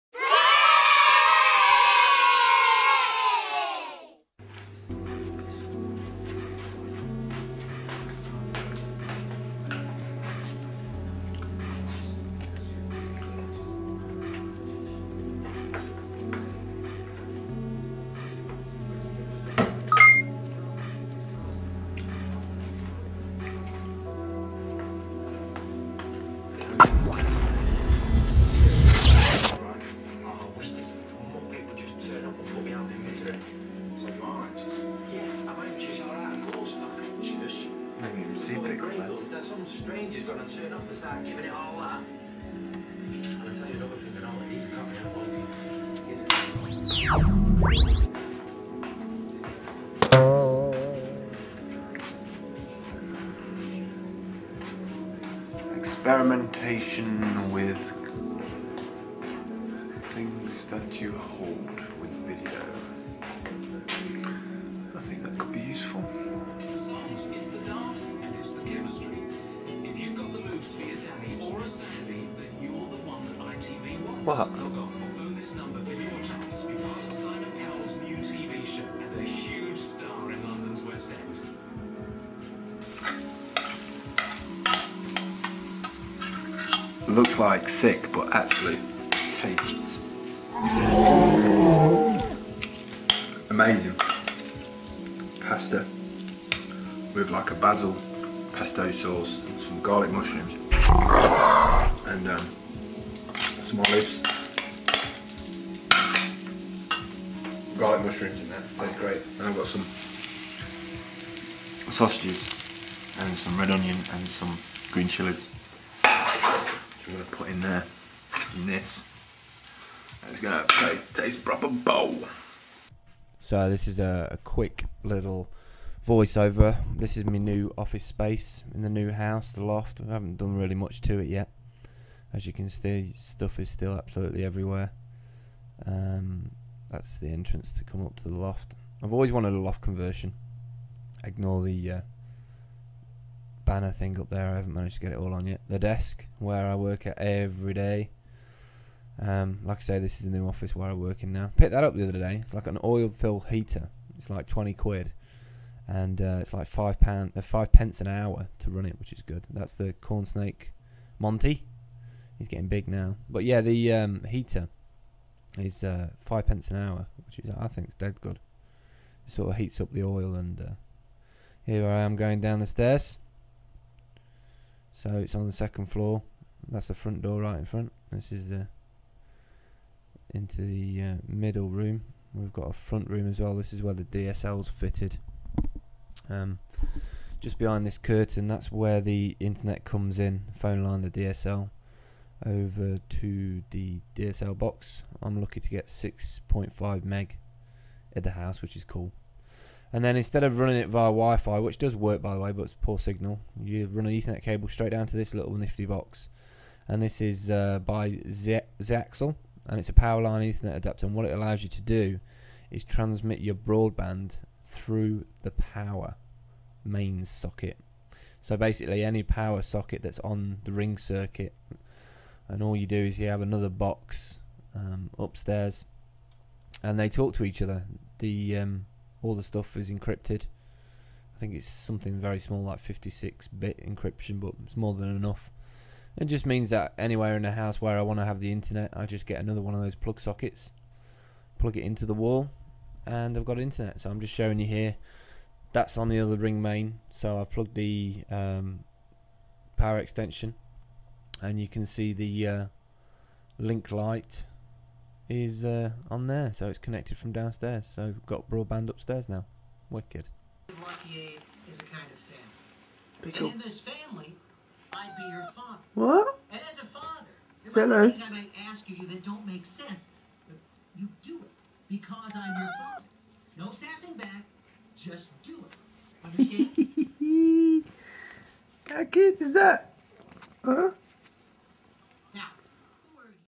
2007 - we are in the new house
Sorry it took so long to get adjusted. I was messing around with my desk setup and the new house move took it out of me there for a while - i've got loads of video that i took over the december period that i will flash back too but my main focus now is working on socialninja videos. I'll still post video here but they will be more "videoblog" diary things - especially with sam expecting our baby in may. :) i’m using